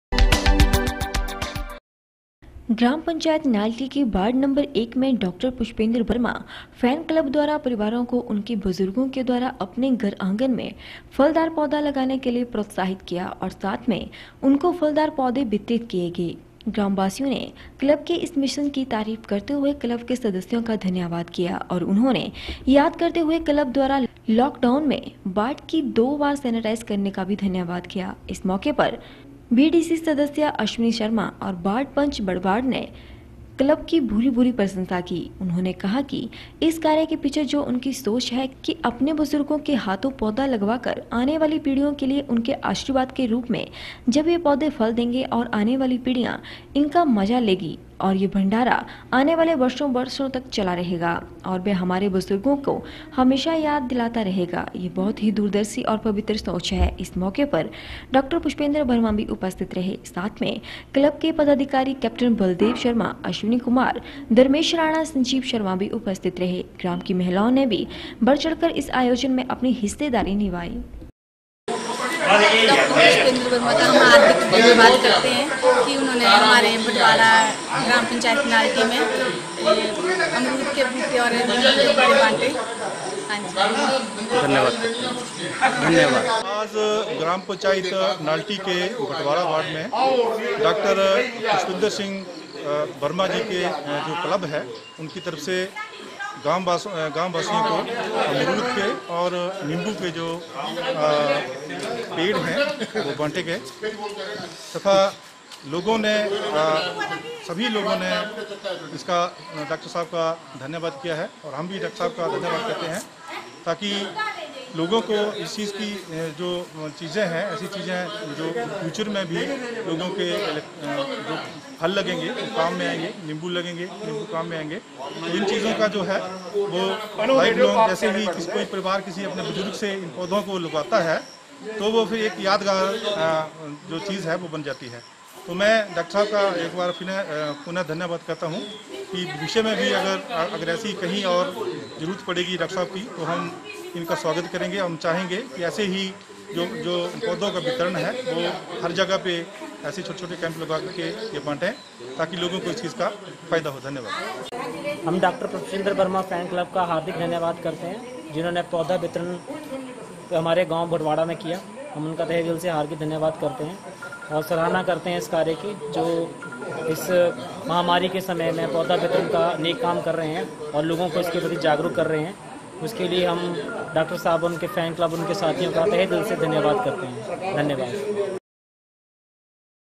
5 aug 10 डॉक्टर पुष्पेंद्र वर्मा फैन क्लब"ने बुजुर्गों के हाथों से लगवाए फलदार पौधे
ग्राम पंचायत नाल्टी  के वार्ड 1में "डॉक्टर पुष्पेंद्र वर्मा फैन क्लब "द्वारा  परिवारों को उनके बुजुर्गों के द्वारा अपने घर आंगन में फलदार पौधा लगाने के लिए प्रोत्साहित किया और साथ में उनको फलदार पौधे वितरित किए गए। ग्राम वासियों ने क्लब के इस मिशन की  तारीफ करते हुए क्लब के सदस्यों का धन्यवाद किया और उन्होँने याद करते हुए क्लब द्वारा लॉ डाउन में वार्ड की 2 बार सेनिटाइज़ करने का भी धन्यवाद किया। इस मौके पर बी डी सी  सदस्य श्री अश्वनी शर्मा जी और वार्ड पंच भडवाड ने क्लब की भूरी भूरी प्रशंशा  की। उन्होंने कहा कि इस कार्य के पीछे जो उनकी सोच है कि अपने बुजुर्गों के हाथों पौधा लगवा कर आने वाली पीढ़ियों के लिए उनके आशीर्वाद के रूप में जब ये पौधे फल देंगे  और आने वाली पीढियाँ  इसका मजा लेंगीं और ये भंडारा आने वाले वर्षों वर्षों तक लगा रहेगा और वह हमारे बुजुर्गों के हमेशा याद दिलाता रहेगा , ये बहुत ही दूरदर्शी और पवित्र सोच है।इस मौके पर डॉक्टर पुष्पेंद्र वर्मा भी उपस्थित रहे।साथ में क्लब के पदाधिकारी   कैप्टन  बलदेव शर्मा,अश्वनी कुमार ,धर्मेश राणा और संजीव शर्मा  भी उपस्थित  रहे। ग्राम की महिलाओं ने भी बढ़-चढ़कर इस आयोजन में अपने हिस्सेदारी निभाई।